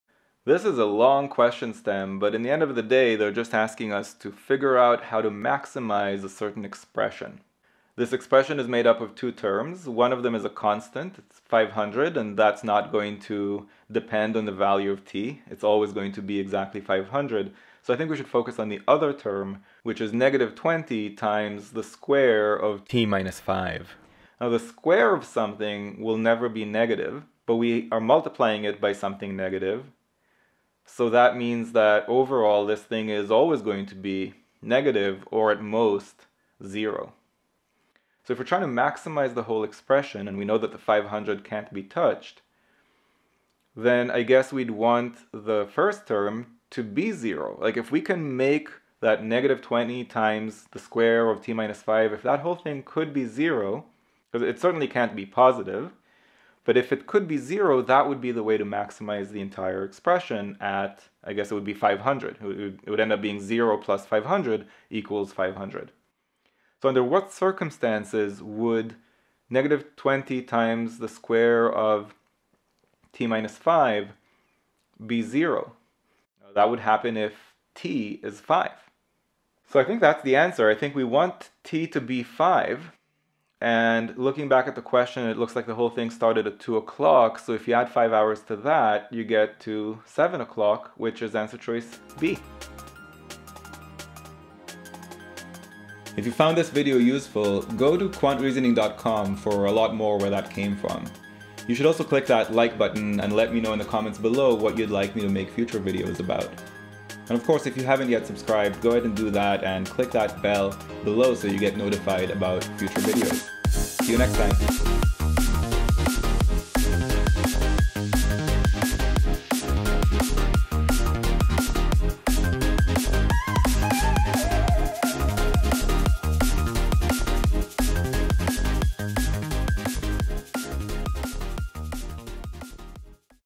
According to a certain estimate, the depth N(t) PS10546
GMAT PS10546

According to a certain estimate, the depth N(t), in centimeters, of the water in a certain tank at t hours past 2:00 in the morning is given by N(t) = −20(t − 5)^2 + 500 for 0 ≤ t ≤ 10. According to this estimate, at what time in the morning does the depth of the water in the tank reach its maximum?